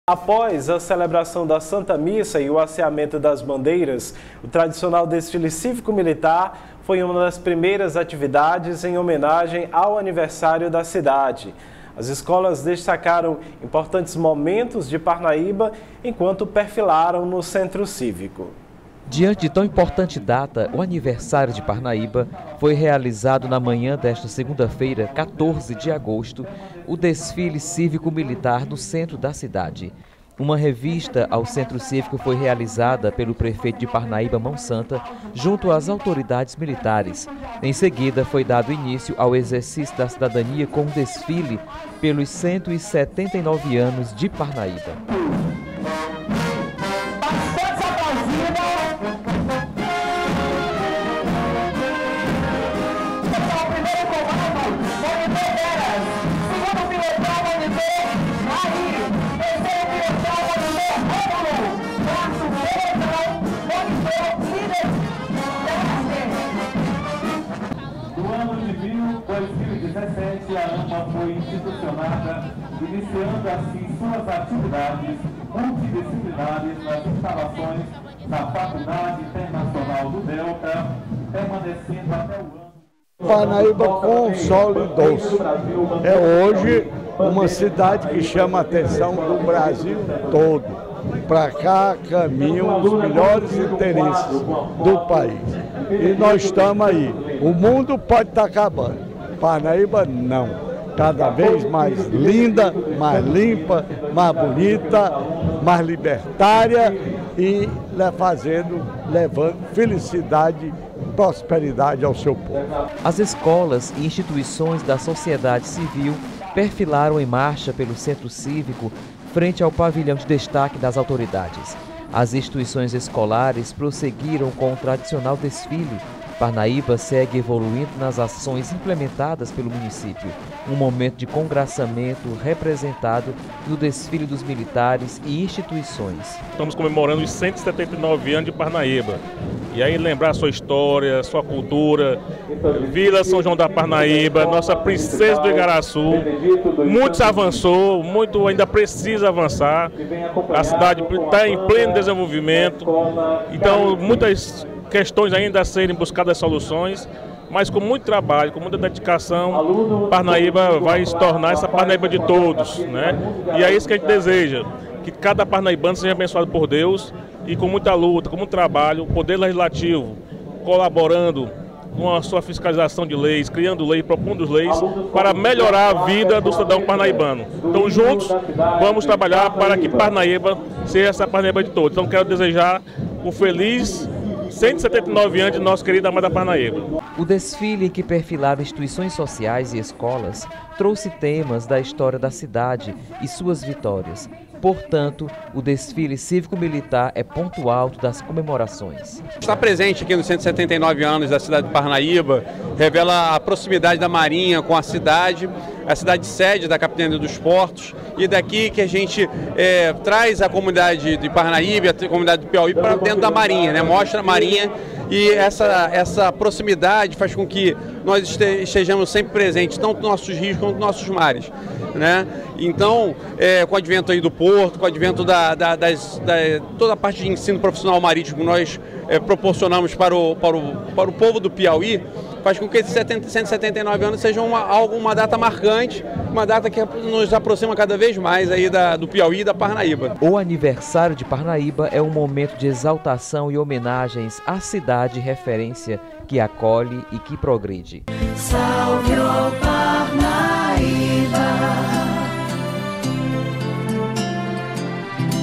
Desfile Cívico-Militar comemora 179 anos de Parnaíba
O tradicional desfile Cívico-militar foi um das primeiras atividades em homenagem ao aniversário da Parnaíba. As escolas destacaram importantes momentos da cidade enquanto perfilaram no centro Cívico.